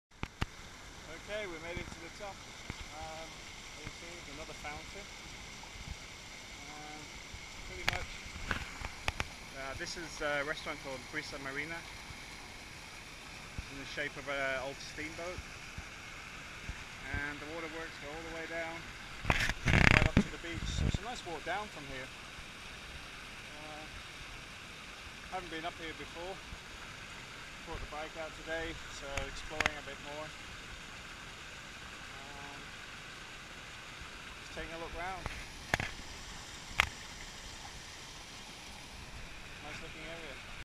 Paraje Natural Molino Del Agua Parc Naturel - La Mata / Torrevieja
The second part of my video as I cycle to Torrevieja, this is me stopping off at the top of the Park in Torre La Mata.